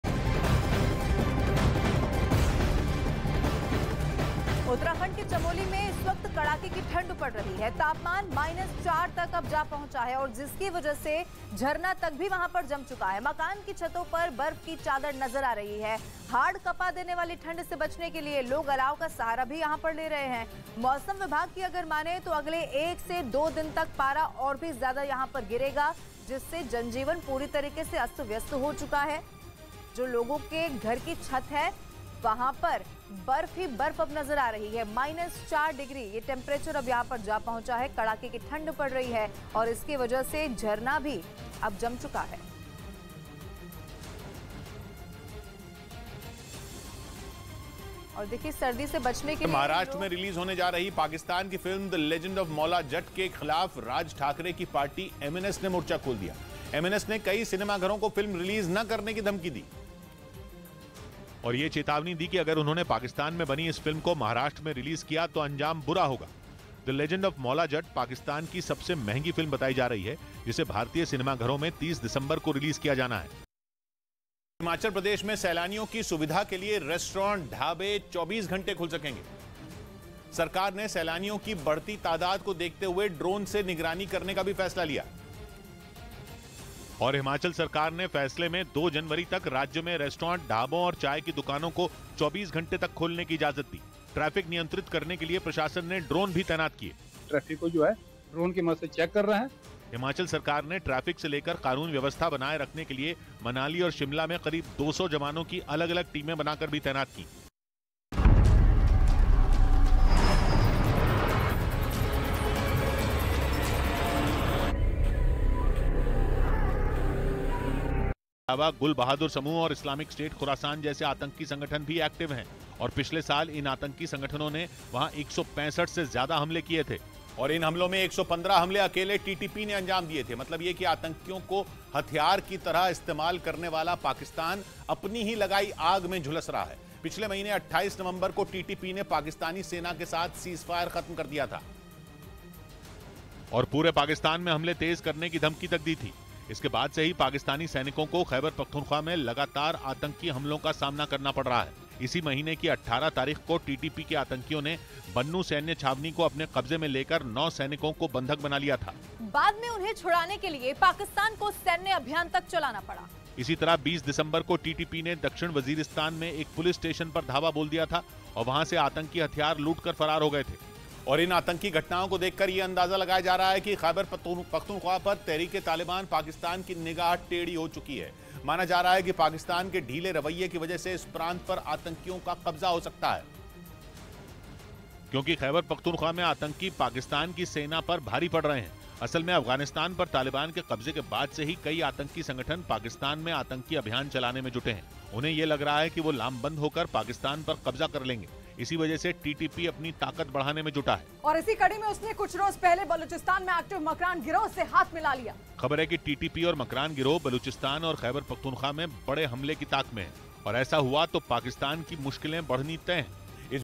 Uttrakhand News: Chamoli में (-)4 तक पहुंचा तापमान, ठंड के कारण जम गया झरना | Hindi News | News18
Uttrakhand News: Chamoli में कड़ाके की ठंड पड़ रही है। तापमान (-)4 तक अब जा पहुंचा है। और जिसकी वजह से झरना तक वहां पर जम गया है। मकान की छतों पर बर्फ़ की चादर नज़र आ रही है। मौरम विभाग के मुताबिक 1-2 दिन तक यहां पारा और ज़्यादा गिरेगा।

It is getting cold in Chamoli. The temperature has now reached (-) 4. And because of which even the waterfall has frozen over there. A sheet of snow is visible on the roofs of the houses. According to the Mauram department, the mercury will drop further here for 1-2 days.